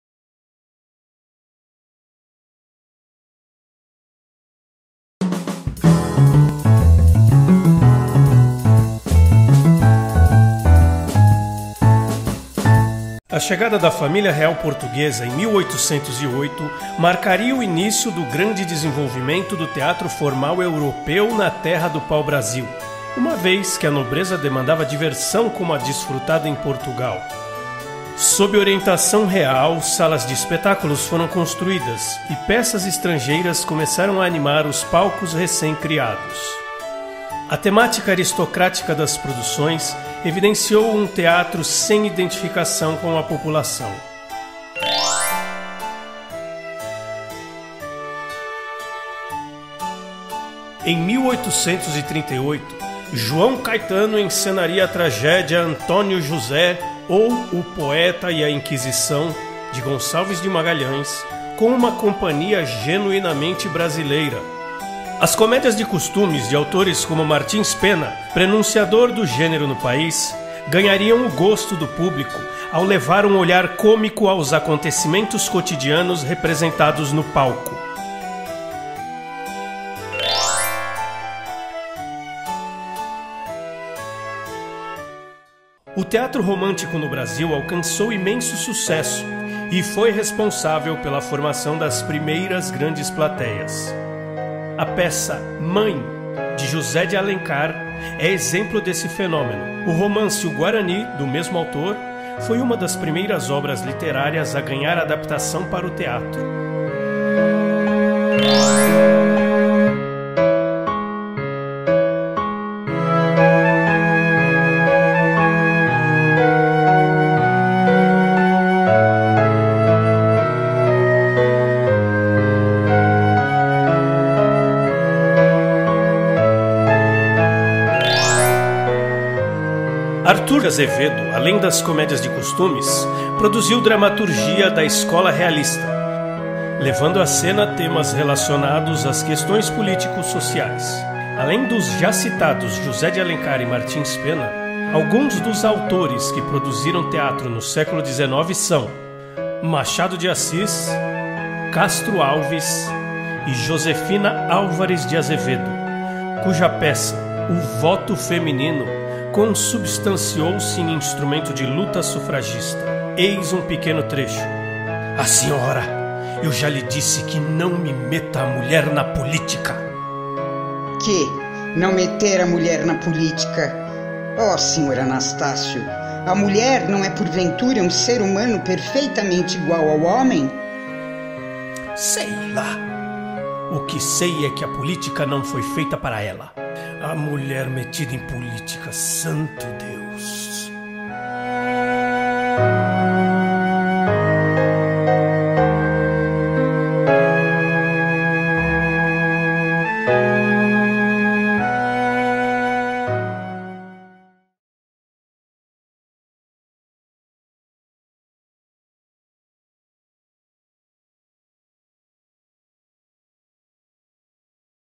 Stories do Teatro | EP 8 | Teatro no Brasil – Século XIX
Neste novo episódio da série "Stories do Teatro", a trupe do Teatro Cartum apresenta o Teatro no Brasil durante o Século XIX.

Com a chegada da família real portuguesa, em 1808, iniciou-se o desenvolvimento teatral formal europeu em nosso país. Sob a orientação real, salas de espetáculos foram construídas e peças estrangeiras começaram a ser encenadas nos palcos recém-criados.

Em 1838, João Caetano encenou a tragédia “O Poeta e a Inquisição”, de Gonçalves de Magalhães, com uma companhia genuinamente brasileira.

Posteriormente, ao levar um olhar cômico aos acontecimentos cotidianos representados no palco, as comédias de costumes ganharam o gosto do público. Também não podemos deixar de mencionar que o Teatro Romântico no Brasil alcançou imenso sucesso e foi responsável pela formação das primeiras grandes plateias.

Está esperando o quê?
Aperte o play e confira o oitavo episódio da série “Stories do Teatro”!